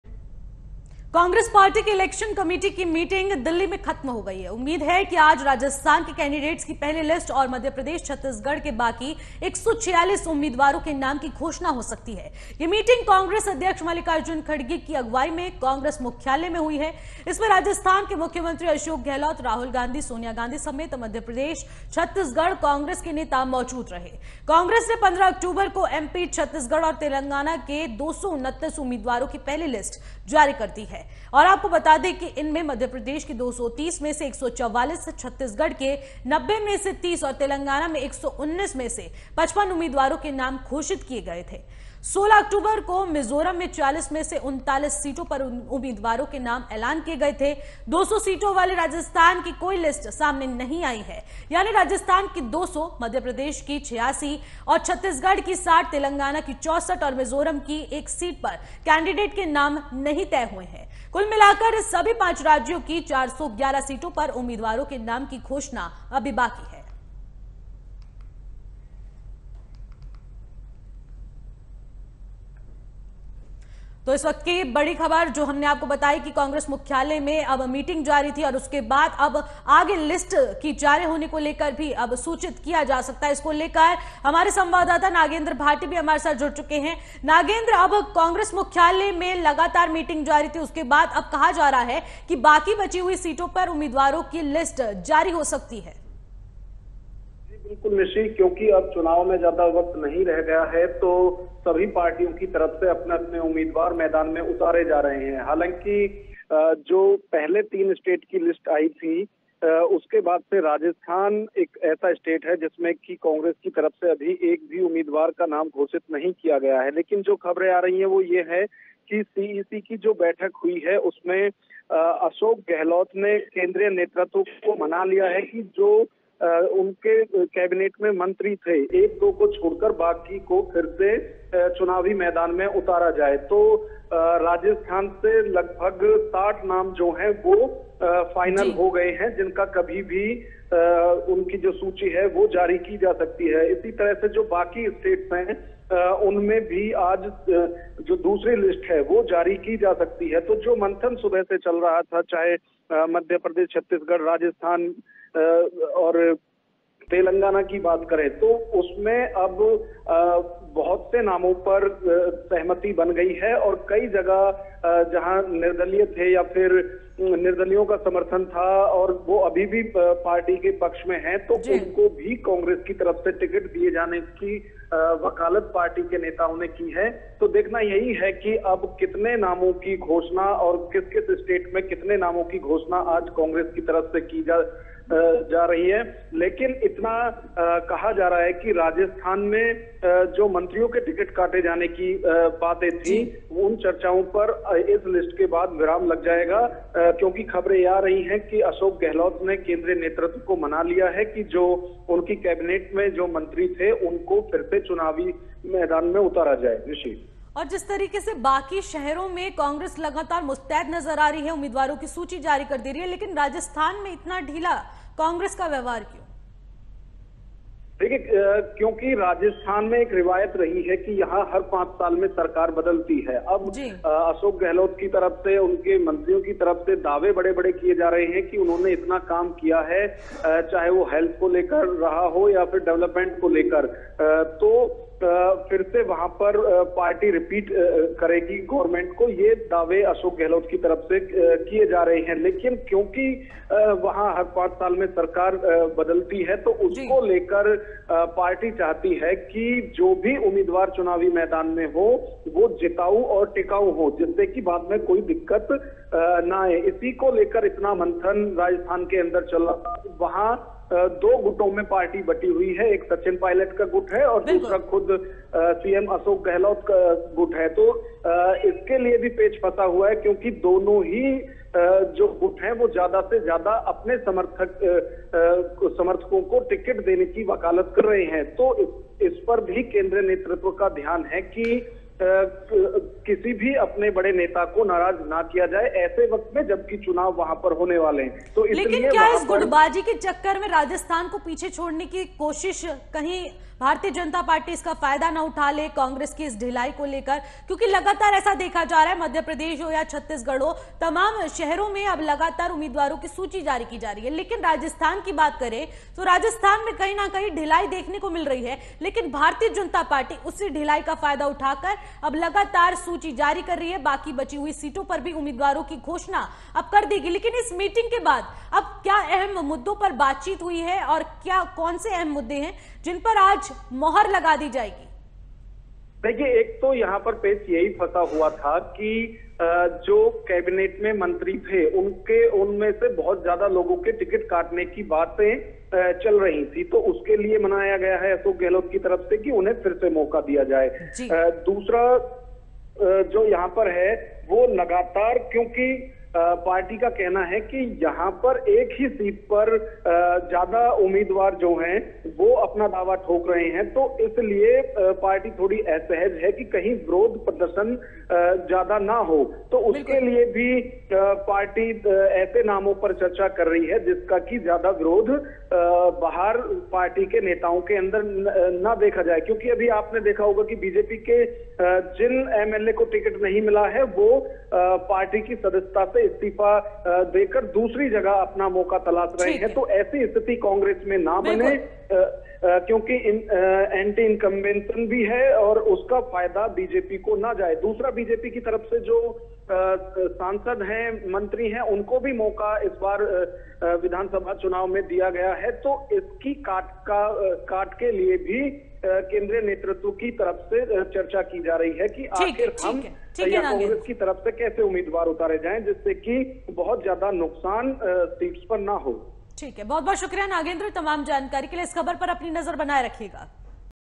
Congress List: Will the rift of Kamalnath And Digvijay Affect State Election? | News Watch India
Congress List: Will the rift of Kamalnath And Digvijay Affect State Election? | News Watch India. Is there a rift brewing between two former Congress chief ministers, Mr Kamal Nath and Mr Digvijay Singh?
A video showing Mr Kamal Nath asking party men to tear clothes of former chief minister Mr Digvijay Singh for denying the party ticket to a local leader, went viral on Tuesday.
It all started after the supporters of Virendra Raghuvanshi started protesting in front of Mr Nath’s residence here for denying him ticket from Shivpuri Assembly constituency.
Mr Raghuvanshi switched over to Congress from BJP, following an assurance that he would be fielded from Shivpuri.

Israel Hamas War: What did Joe Biden and Putin say about Israel Palestine War? | News Watch India